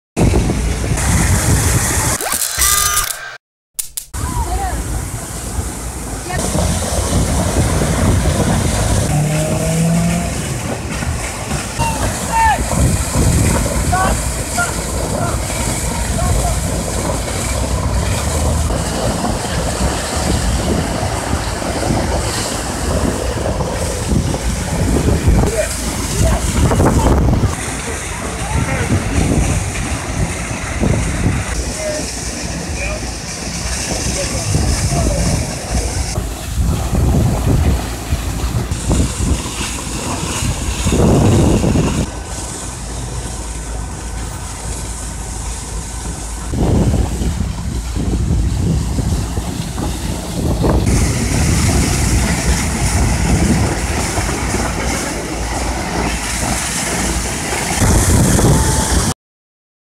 Los Angeles Fire Hydrant Geyser
Footage is from 3:30pm to 4:00pm

I wound up in Los Angeles while out Ubering one day. After dropping off my passenger, I was headed south on S Figueroa St, when I noticed a gushing tower of water ahead. I went offline from Uber, and continued driving towards it for a closer look at what happened. I ended up parking the car at a Ross/Rite Aid parking lot, and recorded clips and photos over the duration of 30 minutes. The water still hadn't been shut off by the time I left, and I have no idea for how long it was going on for before I got there. Talk about a waste of water.